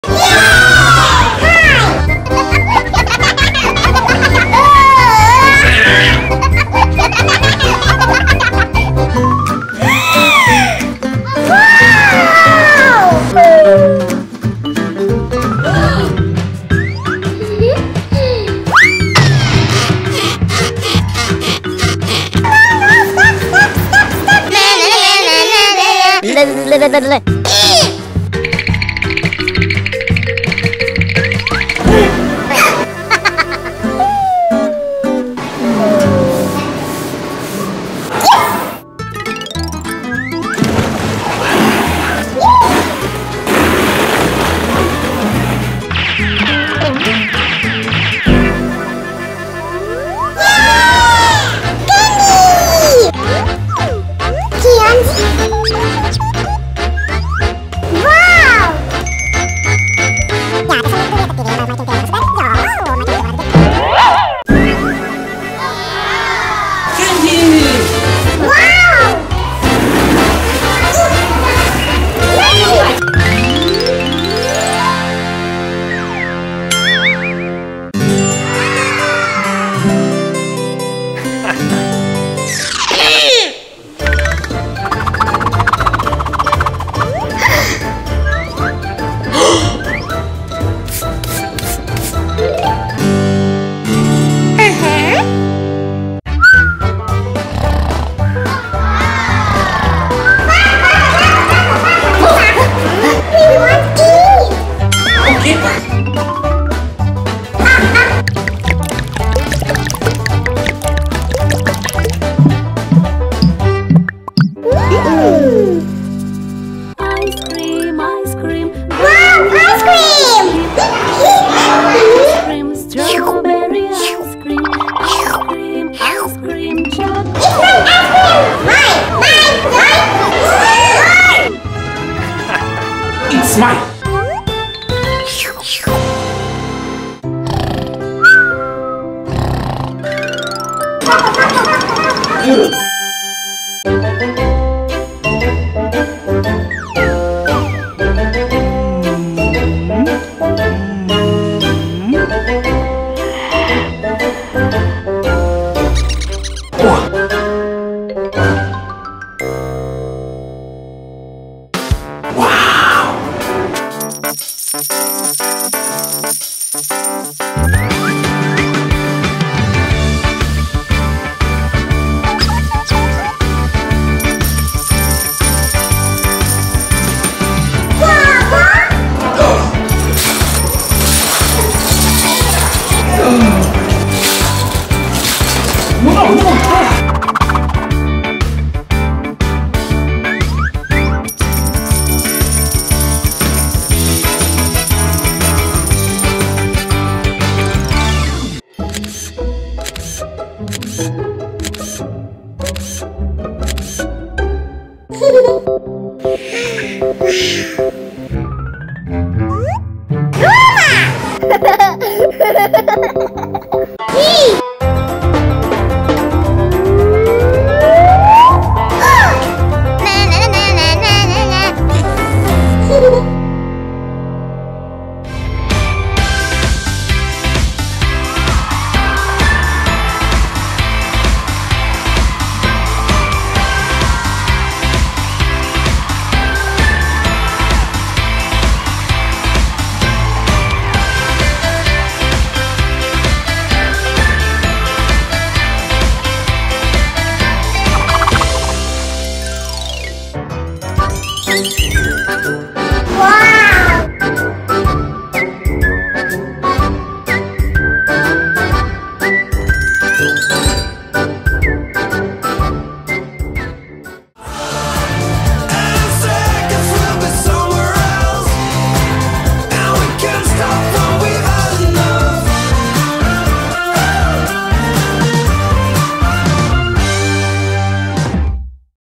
Diana and her Adventure at the Selfie museum in Dubai
Diana and Roma have fun at the Selfie Museum with dad. It was a fun adventure day!